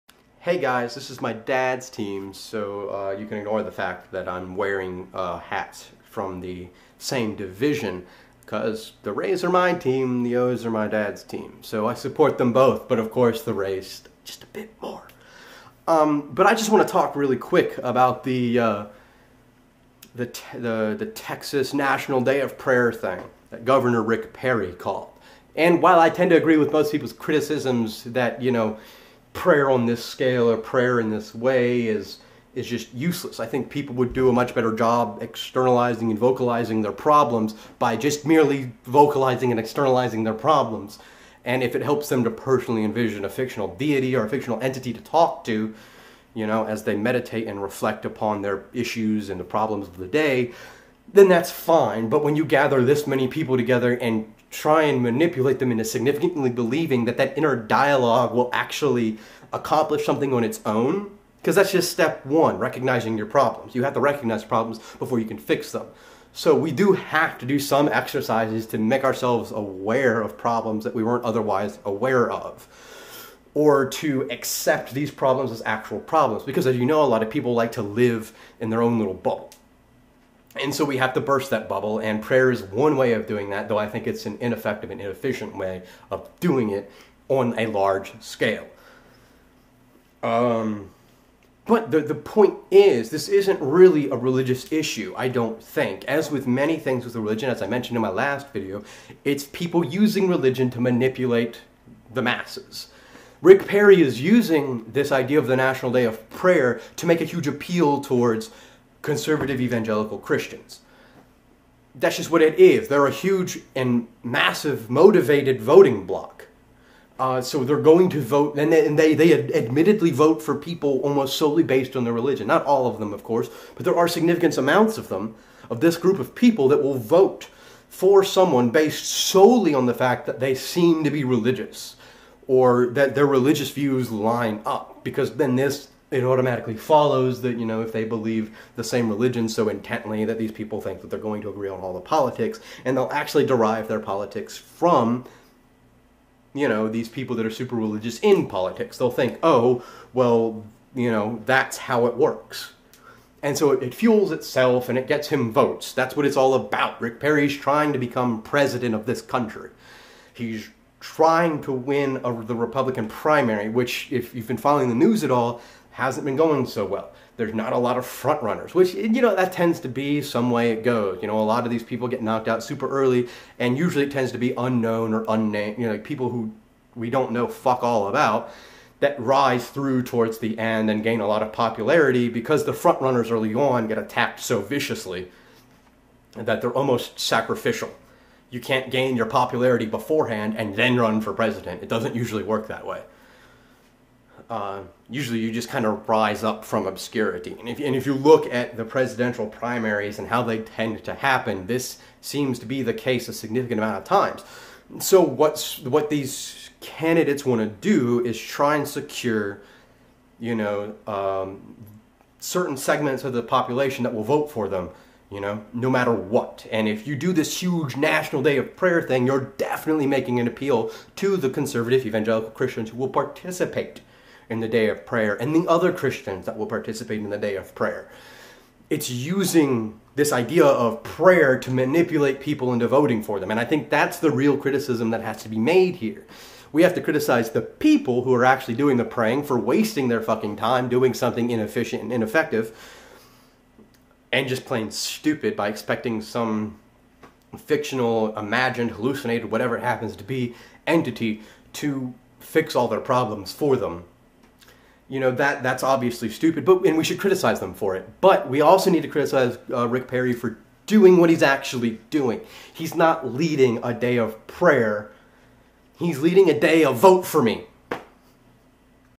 Gov. Rick Perry: Presidential Candidate or Religious Leader?
Gov. Rick Perry's actions are far more political than religious in nature. Religion is merely his preferred tool for manipulating large sectors of society into voting for him rather than other candidates for the sake of gaining more power. Here is a HuffingtonPost article that I found which also touches on this point:

While the general air of criticism contains a lot that I agree with, I haven't seen this as much as I have base criticisms of the religion alone without taking it into the context of the political maneuverings beneath the surface of this massive prayer session.